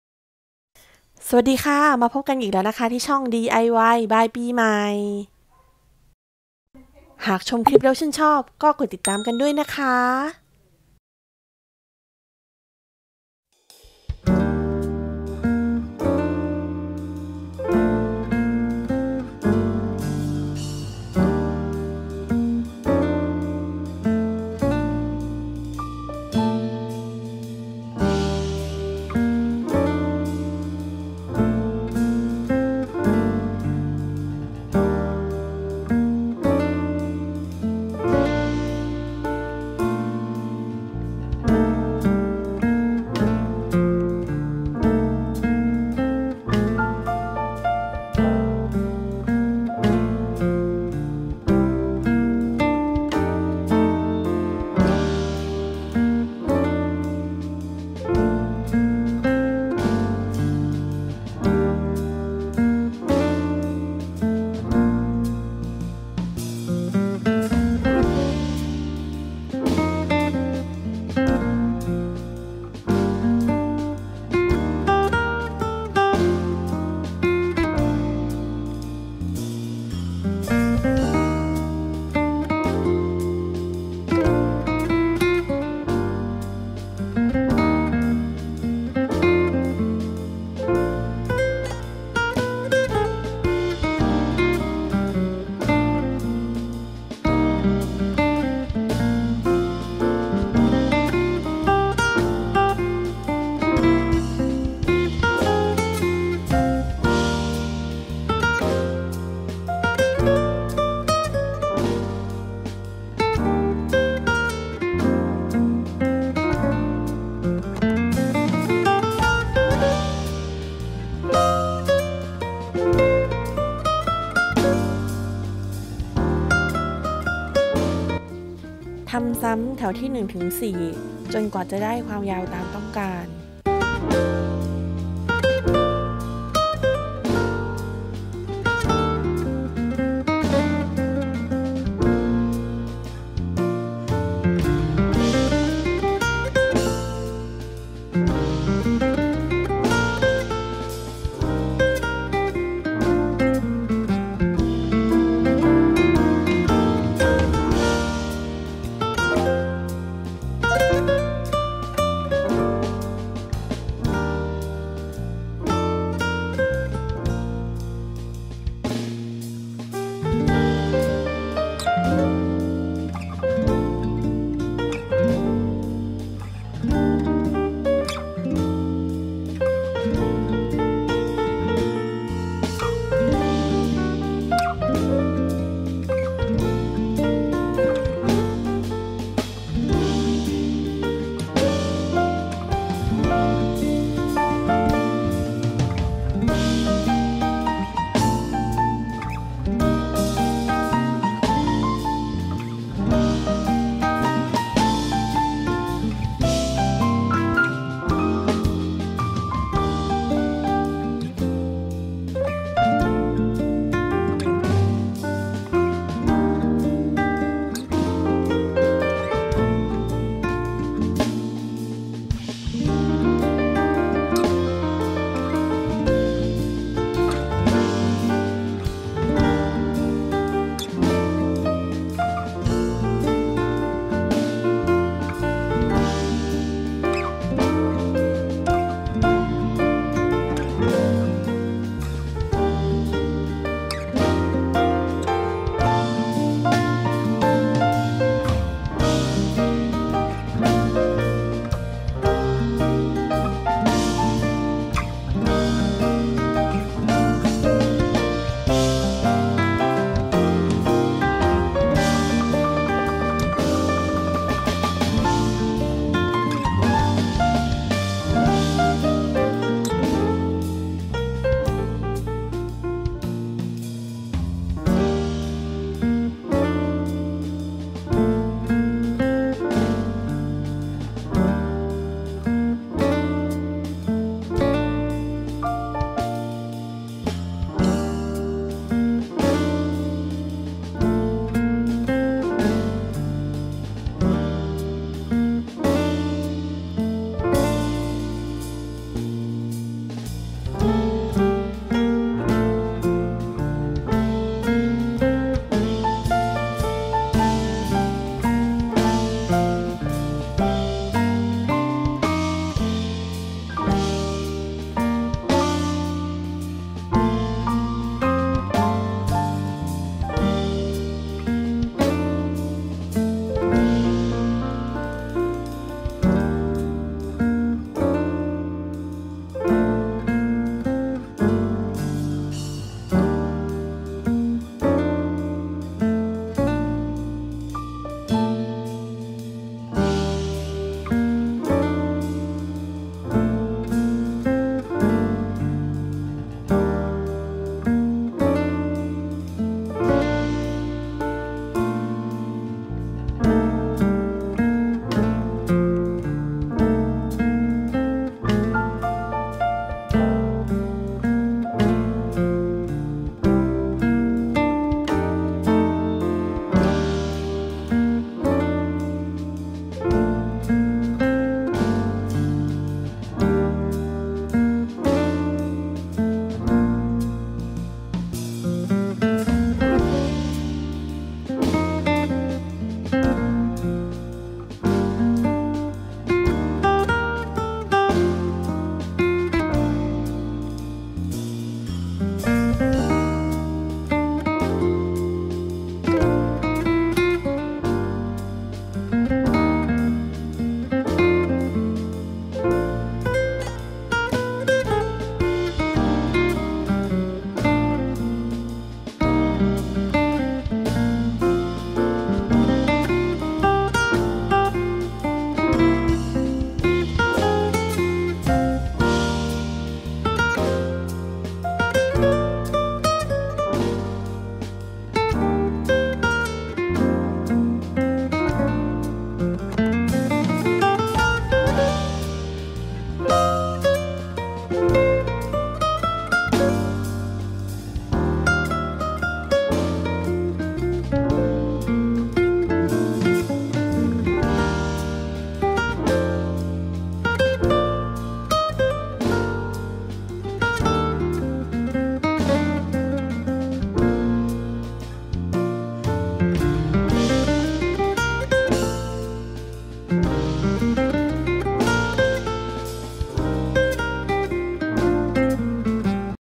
สอนถักสร้อยข้อมือ วิธีการทำง่าย สามารถทำจากไหมปักผ้า ไหมพรม
🌟🌟DIY สอนถ้กสร้อยข้อมือให้กับผู้ที่สนใจ อธิบายขั้นตอนการทำละเอียด โดยแสดงวิธืการทำง่ายๆ สามารถทำตามได้ ถักใส่เองก็ภูมิใจ ให้เป็นของขว้ญก็ภูมิใจทั้งผู้ให้และผู้รับ
  🌈🌈เพียงมีใจรักในงานฝีมือ งาน DIY ต้องการทำขึ้นมาด้วยตัวเอง เมื่อทำสำเร็จไดรับความรู้สึกถึงความตั้งใจ และความใส่ใจ มาร่วมทำอะไร สนุก และชิลล์ไปด้วยกัน ❤️🧡💛💚💙

1. ไหมปักผ้า /ไหมพรม
2.  กรรไกร
3. ไม้บรรทัด